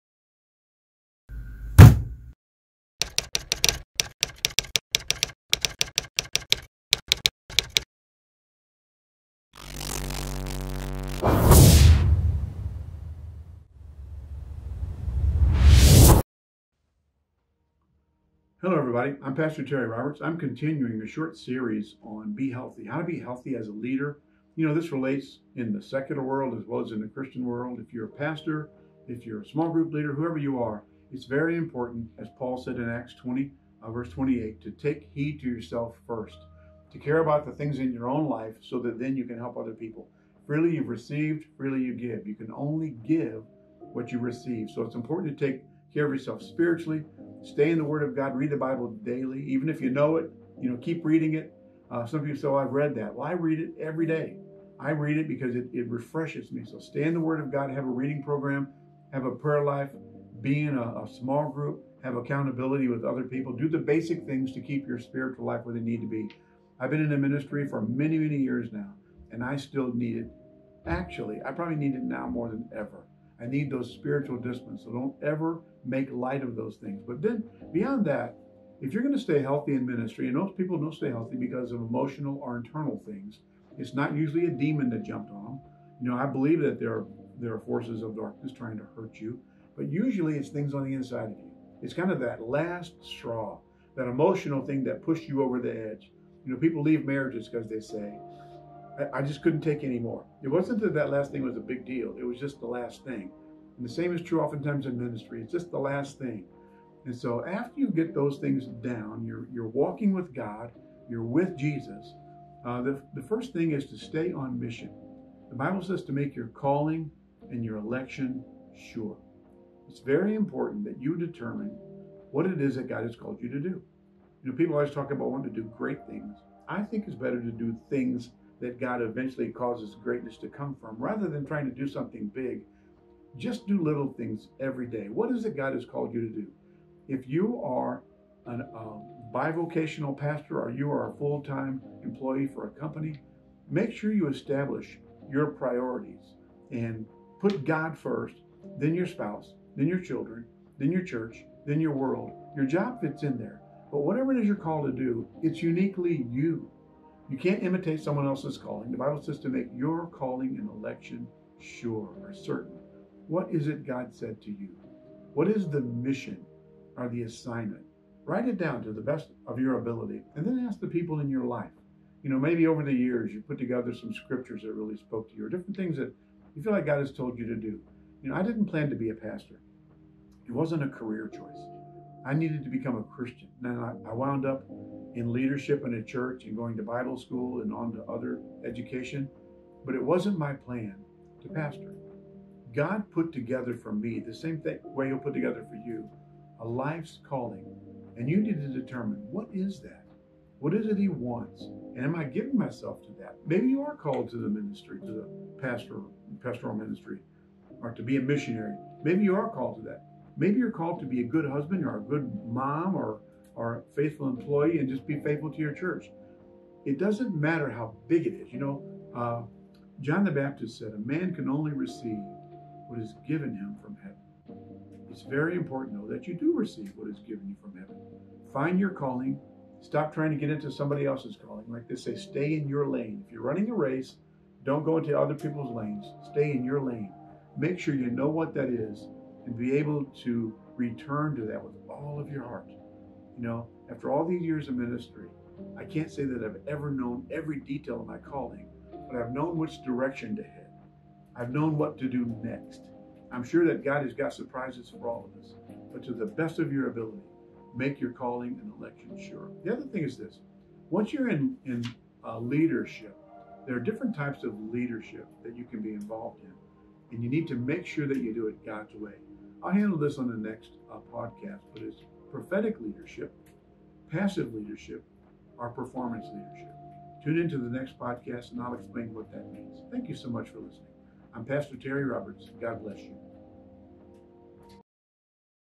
The Healthy Christian Life | Part 2 | Pastor Terry Roberts
Pastor Terry Roberts expounds on how to live as a healthy christian!